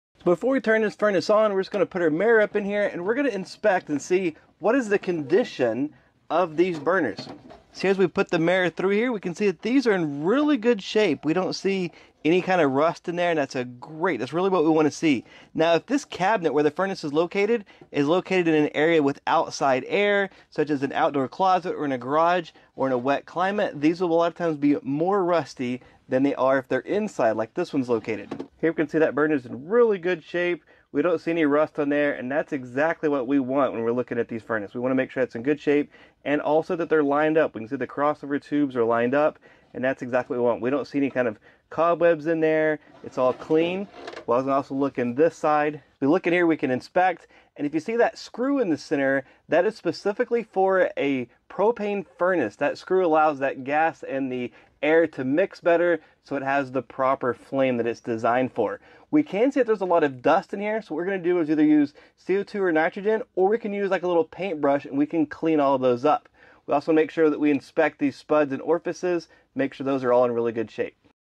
Ty Branaman Covers Properly Inspecting Inshot Burners on a Gas Furnace
In this HVAC Training Video, @love2hvac covers how to inspect in shot burners on a gas furnace! He gives a close up view on the burners while going over what to look for in the field!! Supervision is needed by a licensed HVACR Tech while performing tasks as Experience and Apprenticeship garners Wisdom and Safety.
Our HVACR Educational Books, E-Books, Workbooks, Cards, Posters, and PowerPoints:

Refrigerant Charging Paperback on Amazon